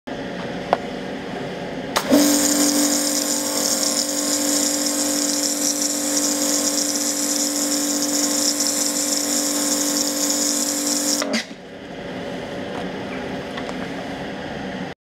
Nano Skin 3 weeks after application
Nano Skin sealant 3 weeks after application on a daily driven car.